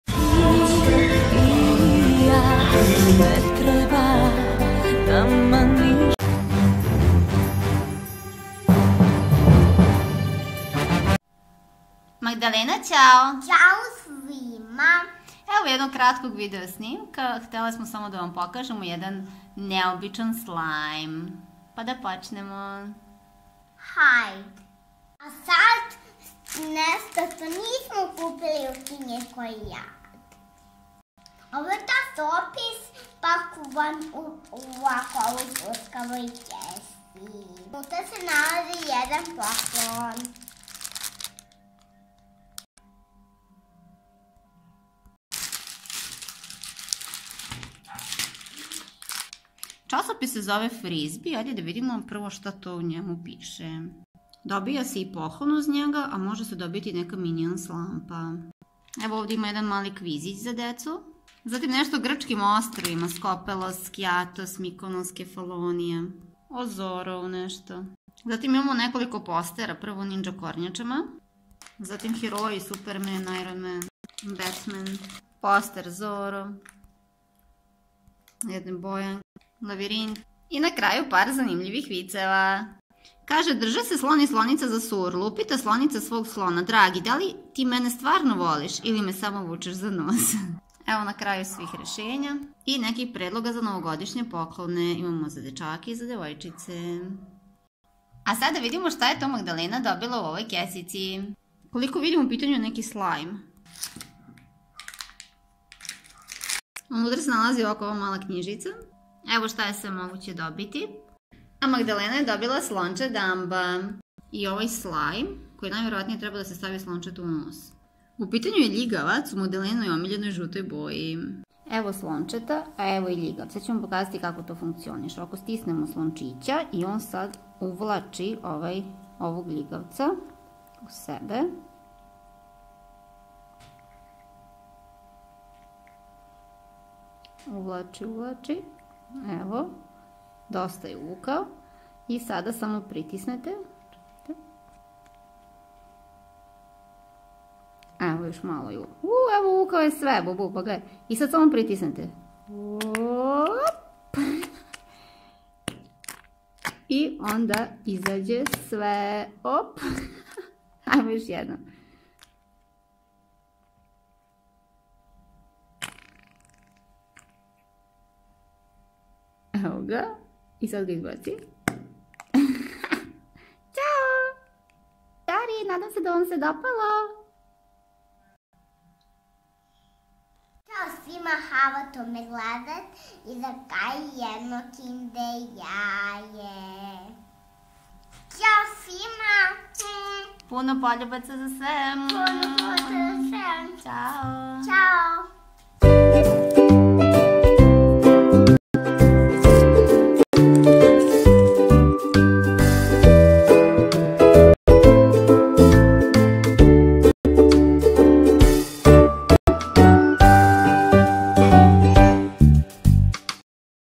Moj novi slime
Dragi drugari nadam se da će Vam se dopasti Magdalenin novi slime, ona je oduševljena ☺ pogledajte i zanimljiv video naše drage Anite na sledećem linku :